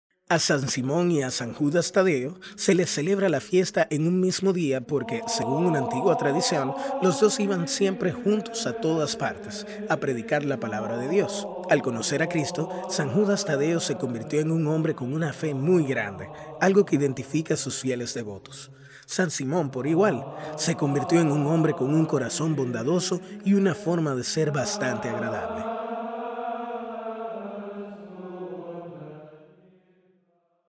San Simón y San Judas Tadeo
Hoy 28 de Octubre, la Iglesia conmemora el Santoral de San Simón y San Judas Tadeo